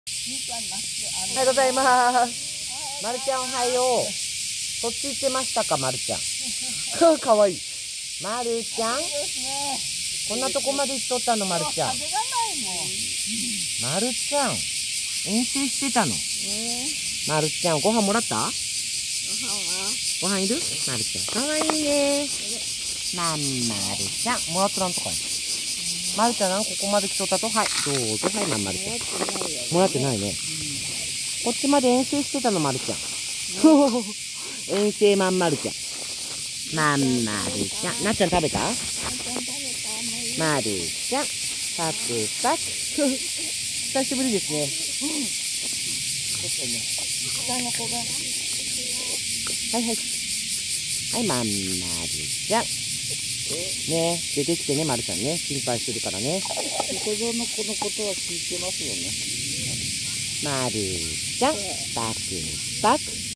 遠征してたまん丸ちゃん(笑)❗こちらの方で寝てたみたい😅‼️いつもの場所に出てこないと心配しますよ(笑)❗寝起きでも直ぐに食べるまん丸ちゃんなんだニャン❤️
遠征してたまん丸ちゃん(笑)❗
こちらの方で寝てたみたい😅‼️
いつもの場所に出てこないと心配しますよ(笑)❗
寝起きでも直ぐに食べるまん丸ちゃんなんだニャン❤️

Tamanmaru-chan went on an expedition (laughs)❗
 It looks like I was sleeping here 😅!!  ️
 I'll be worried if you don't come out to the usual place (laughs)❗
 It's Manmaru-chan who eats right after waking up, nyan❤️